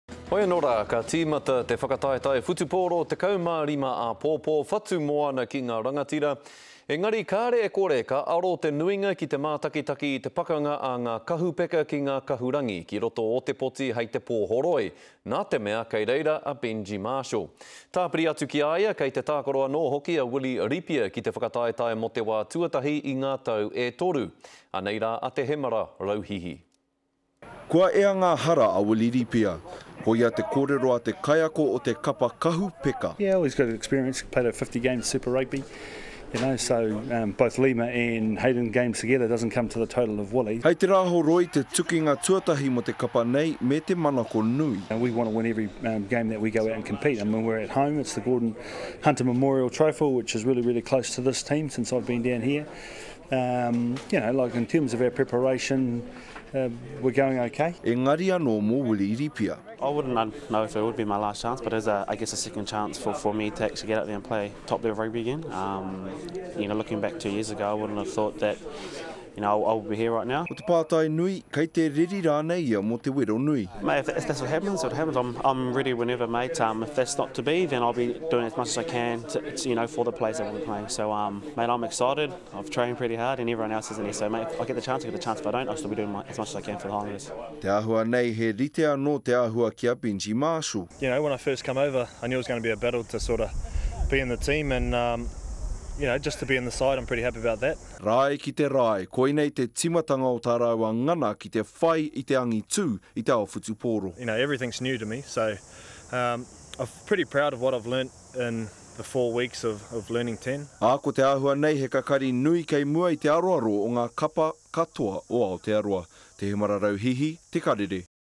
Rugby: Eyes on Willie Ripia and Benji Marshall
Super Rugby kicks off this weekend and the big match on Saturday sees the Highlanders take on the Blues at home. Much talk has focussed around Blues recruit Benji Marshall, but as Te Hemara Rauhihi reports, the Highlander's new-old recruit, Willie Ripia, is set to make his return after a three year absence.